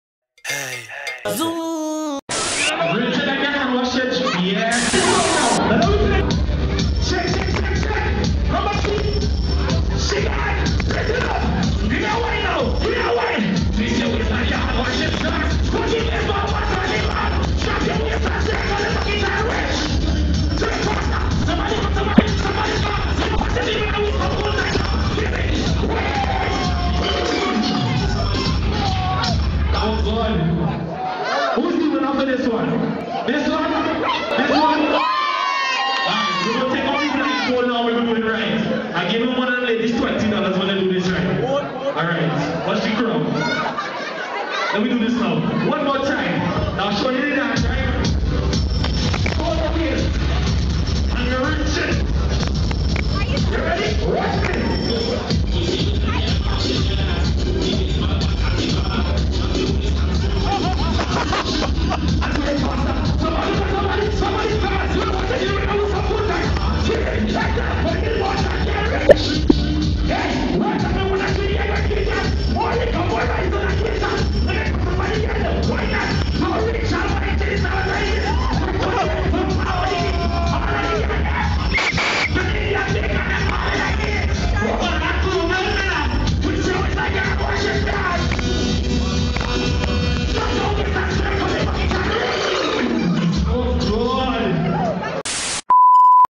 RINCH Sstyles S.A.R.S. 6 [Opening For Lyrikal]
"RINCH" Live Performance via Sstyles
    Along Side  Mr. Cloud 9 "Lyrikal"
For Sars6
                 Nov.15th 2014
Via